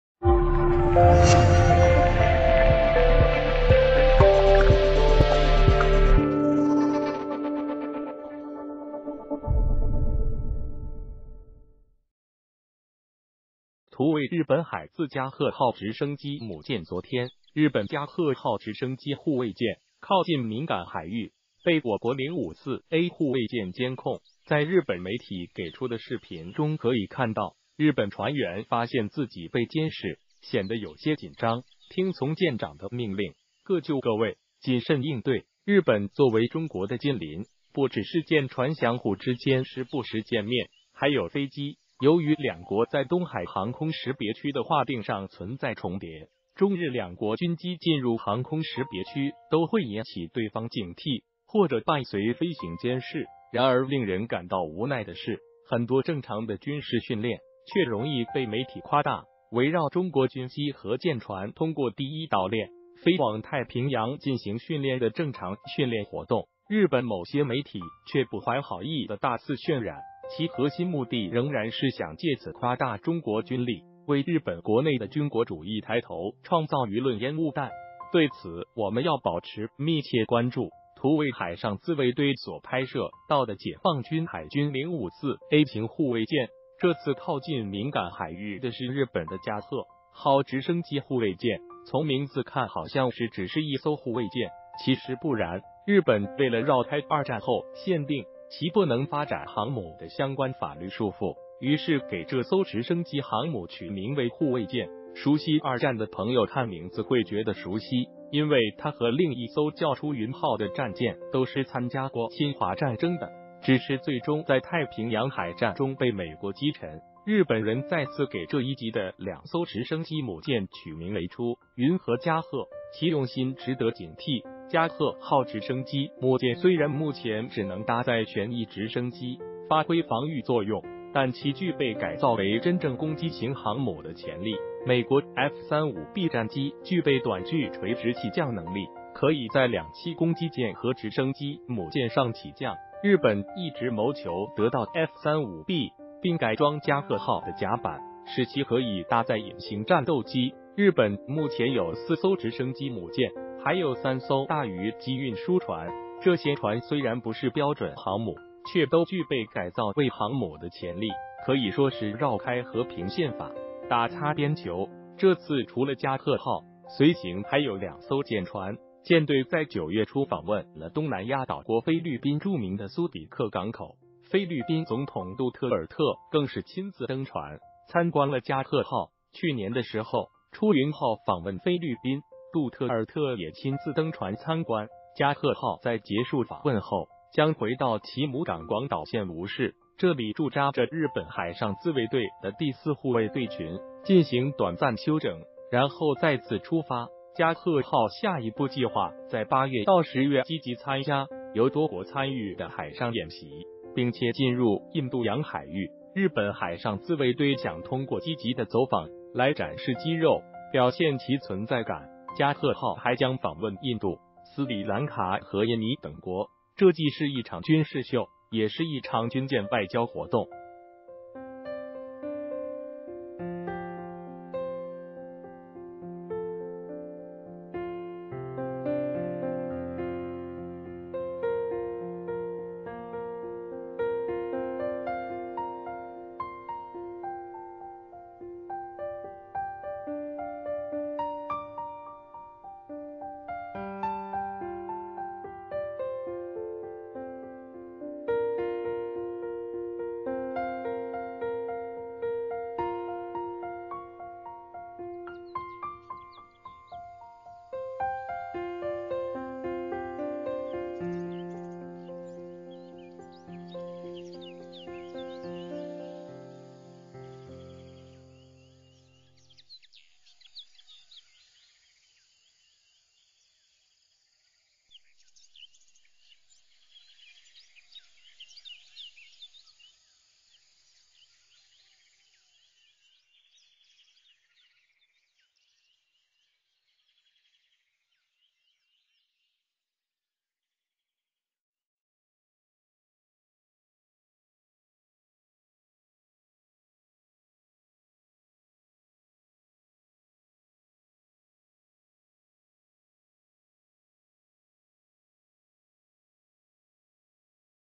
心虚了？日本准航母接近我国海域遭054A监控，操作人员手忙脚乱
谢谢！图为日本海自加贺号直升机母舰昨天，日本“加贺”号直升机护卫舰，靠近敏感海域，被我国054A护卫舰监控。在日本媒体给出的视频中可以看到，日本船员发现自己被监视，显得有些紧张，听从舰长的命令，各就各位，谨慎应对。日本作为中国的近邻，不只是舰船相互之间时不时见面，还有飞机。由于两国在东海航空识别区的划定上存在重叠，中日两国军机进入航空识别区都会引起对方警惕或者伴随飞行监视。然而令人感到无奈的是，很多正常的军事训练，却容易被媒体夸大。围绕中国军机和舰船通过第一岛链，飞往太平洋进行训练的正常训练活动，日本某些媒体却不怀好意的大肆渲染，其核心目的仍然是想借此夸大中国军力，为日本国内的军国主义抬头创造舆论烟雾弹。对此我们要保持密切关注。图为海上自卫队所拍摄到的解放军海军054A型护卫舰这次靠近敏感海域的是日本的“加贺”号直升机护卫舰。从名字看好像是只是一艘护卫舰，其实不然，日本为了绕开二战后限定其不能发展航母的相关法律束缚，于是给这艘直升机航母取名为“护卫舰”。熟悉二战的朋友看名字会觉得熟悉，因为它和另一艘叫“出云”号的战舰都是参加过侵华战争的，只是最终在太平洋海战中被美国击沉。日本人再次给这一级的两艘直升机母舰取名为“出云”和“加贺”，其用心值得警惕。“加贺”号直升机母舰虽然目前只能搭载旋翼直升机，发挥防御作用。但其具备改造为真正攻击型航母的潜力。美国F35B战机具备短距垂直起降能力，可以在两栖攻击舰和直升机母舰上起降。日本一直谋求得到F35B，并改装加贺号的甲板，使其可以搭载隐形战斗机。日本目前有4艘直升机母舰，还有三艘大隅级运输船。这些船虽然不是标准航母，却都具备改造为航母的潜力。可以说是绕开和平宪法，打擦边球。这次除了加贺号，随行还有两艘舰船。舰队在9月初访问了东南亚岛国菲律宾著名的苏比克港口。菲律宾总统杜特尔特更是亲自登船，参观了“加贺”号。去年的时候，“出云”号访问菲律宾，杜特尔特也亲自登船参观。加贺号在结束访问后，将回到其母港广岛县吴市。这里驻扎着日本海上自卫队的第四护卫队群，进行短暂休整，然后再次出发。“加贺”号下一步计划在8月到10月积极参加由多国参与的海上演习，并且进入印度洋海域。日本海上自卫队想通过积极的走访，来展示肌肉，表现其存在感。“加贺号”还将访问印度，斯里兰卡和印尼等国，这既是一场军事秀，也是一场军舰外交活动。